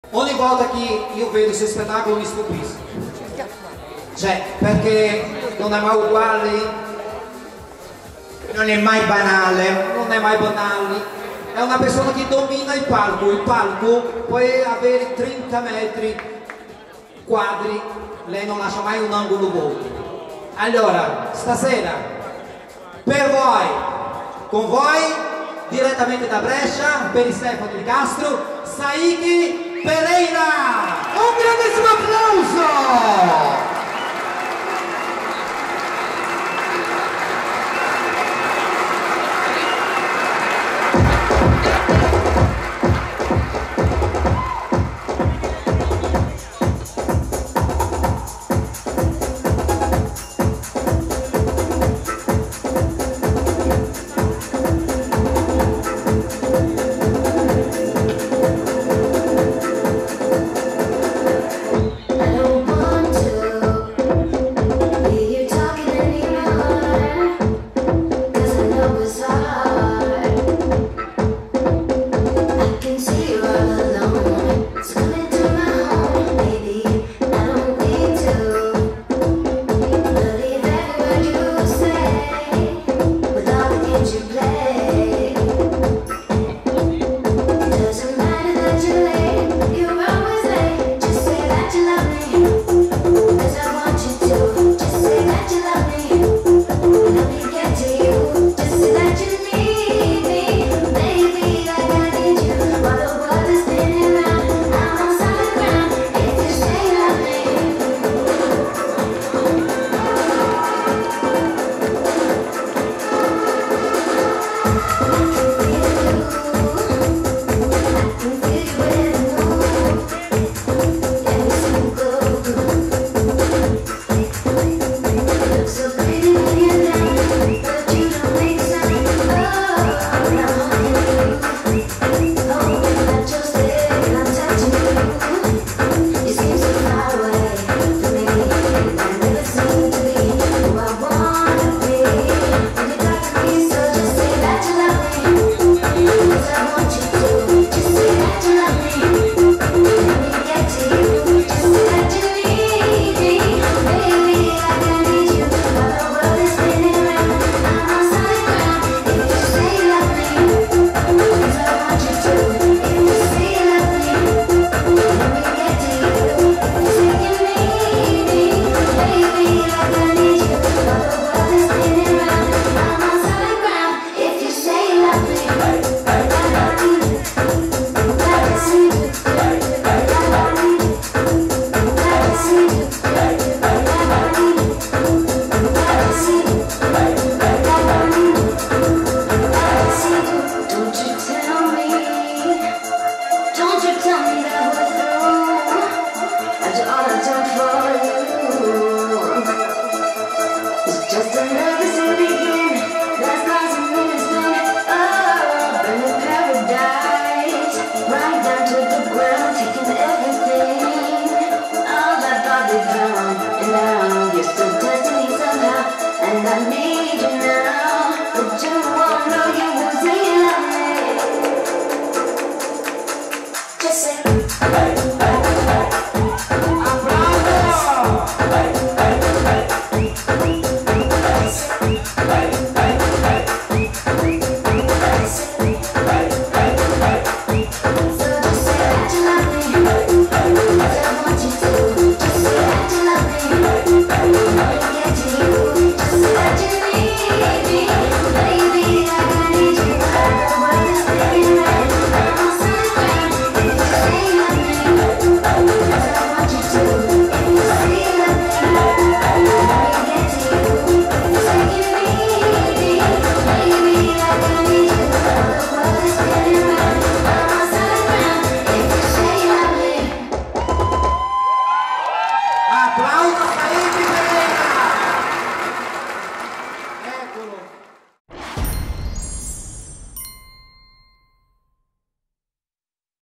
Spettacolo di Sayck Pereira al Compleanno di Stephany De Castro presso Baraonda Club
Svolto il giorno 10 Dicembre 2022, al Baraonda Club Via Alcide de Gasperi, 14 – Milano